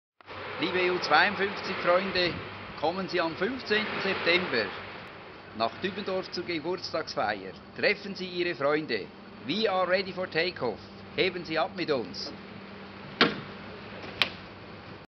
We are ready for take off!
Ju-Air feiert ihren 30. Geburtstag am 15. September 2012 ab 11 bis ca. 18 Uhr

Kommen Sie vorbei - Heben Sie mit uns ab - we are ready for take off!